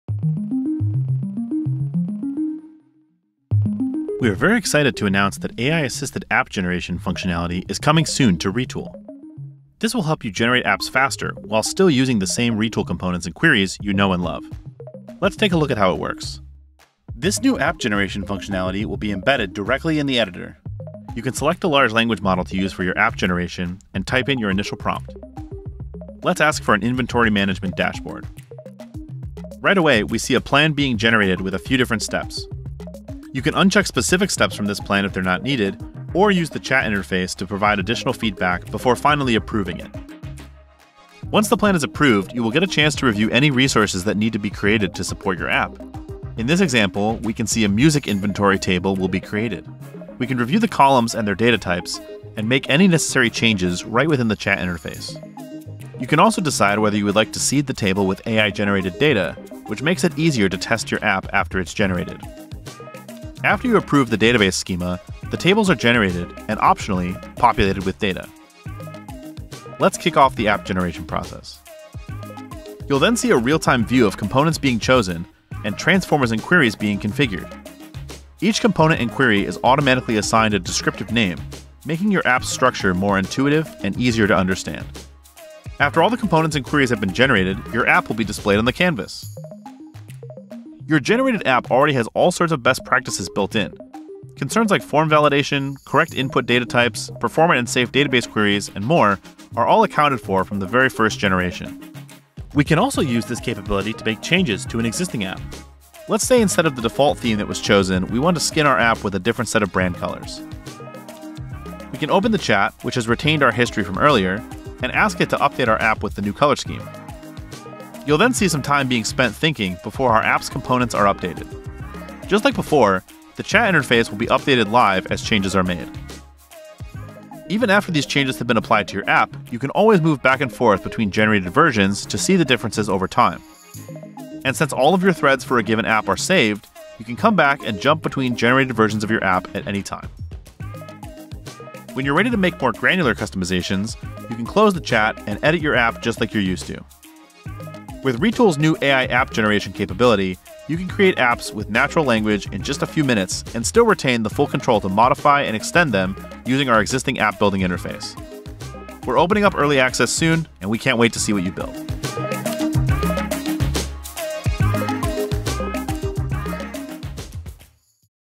Introducing AI-assisted app generation in Retool
We’re very excited to announce that AI-assisted app generation functionality is coming soon to Retool. This will help you generate apps faster while still using the same Retool components and queries you know and love.

We're looking for thoughtful partners who can help us refine this new capability before its broader release. If you'd like to join the wait list, please sign up

00:00 Intro
00:14 Generating an app from scratch
01:42 Modifying an existing app
02:27 Wrap up